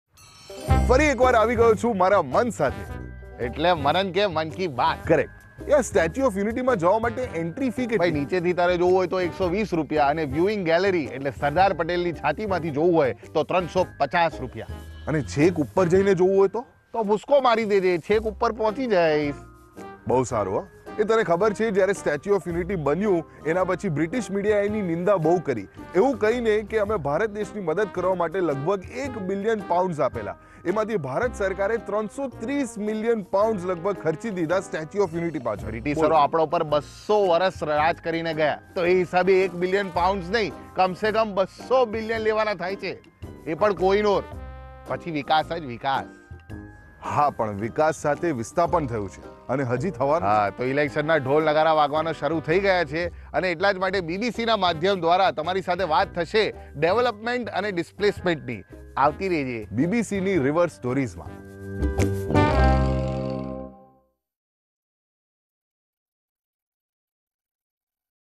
BBCRiverStories સ્ટેન્ડઅપ કૉમેડિયન મનન દેસાઈ કરશે સ્ટેચ્યૂ ઑફ યુનિટી અને નર્મદા નદીની વાત
નર્મદા, સ્ટેચ્યૂ ઑફ યુનિટી, સરદાર સરોવર ડેમ અને ગુજરાતનાં ગામડાંના વિકાસ અને વિસ્થાપનની વાતો સ્ટેન્ડઅપ કૉમેડિયન મનન દેસાઈ અને બીબીસી ગુજરાતી સાથે. ગુજરાતના નર્મદાના કાંઠાના ગામડાંની સ્થિતિ કેવી છે? ત્યાં કેવો વિકાસ છે અને શું છે વિસ્થાપનની સ્થિતિ. આ તમામ માહિતી આ સ્ટોરીઝમાં હશે.